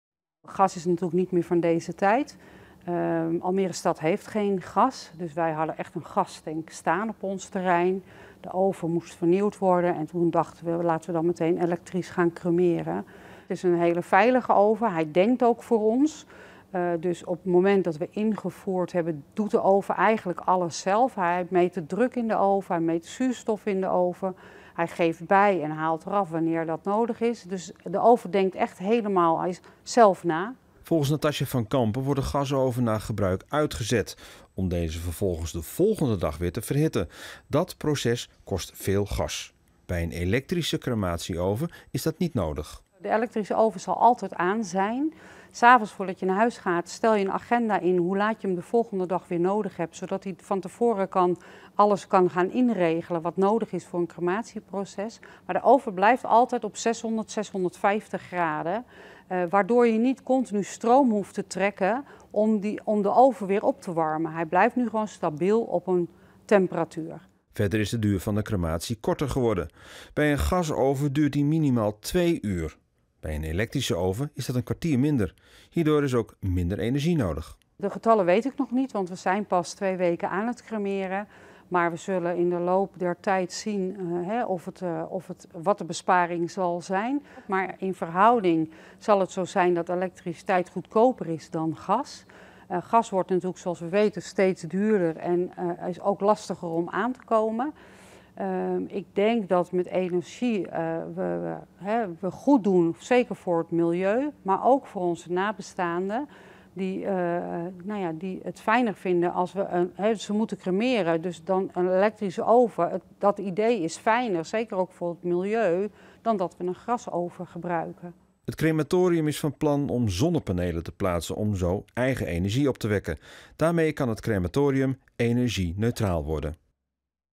Almeers crematorium in de Kruidenwijk is van het gas af
Het crematorium in de Almeerse Kruidenwijk gebruikt geen gas meer om te cremeren. De oude gasoven is vervangen door een elektrisch exemplaar. Deze is minder milieubelastend en energiezuiniger dan een gasgestookte oven.